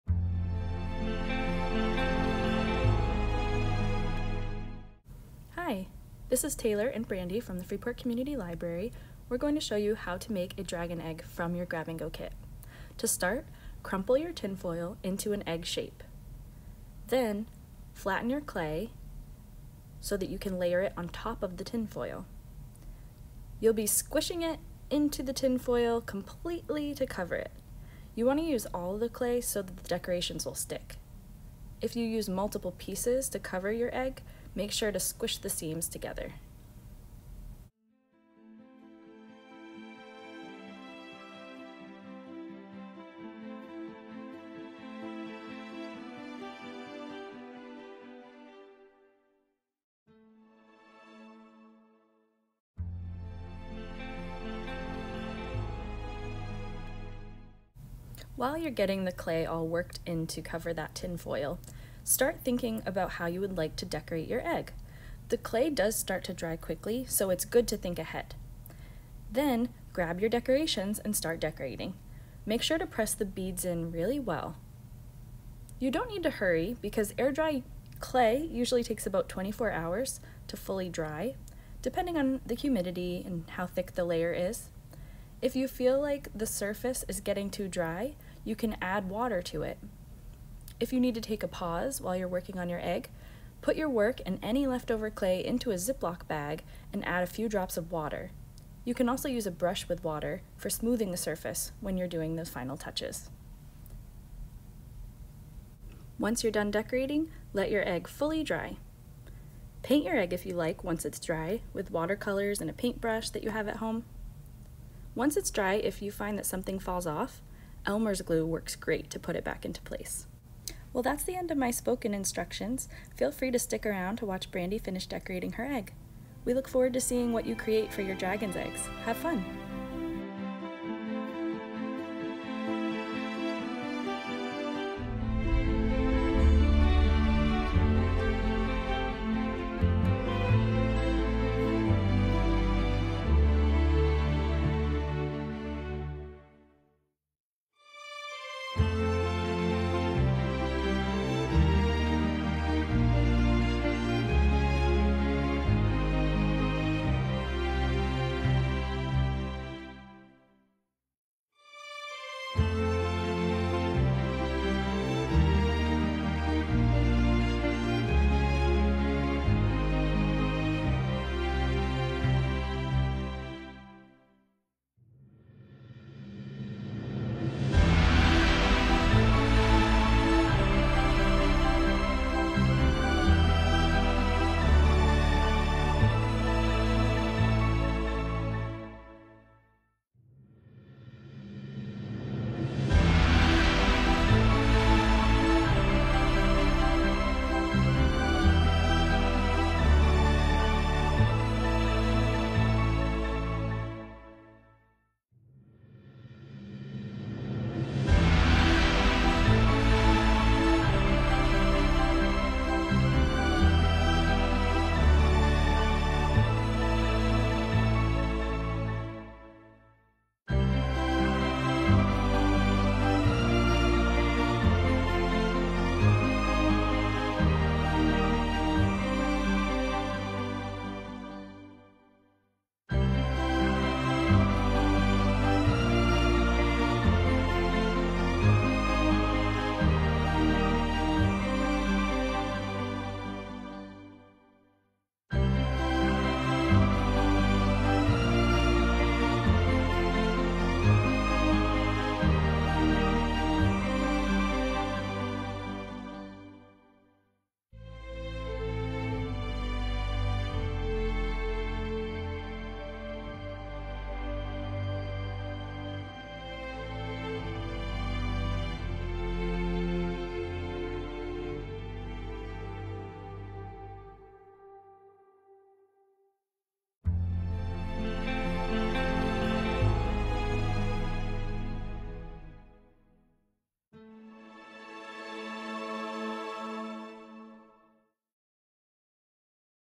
Dragon Egg: A Grab-and-Go Craft
FCL staff shows how to create a dragon egg using tin foil, clay, and shiny beads!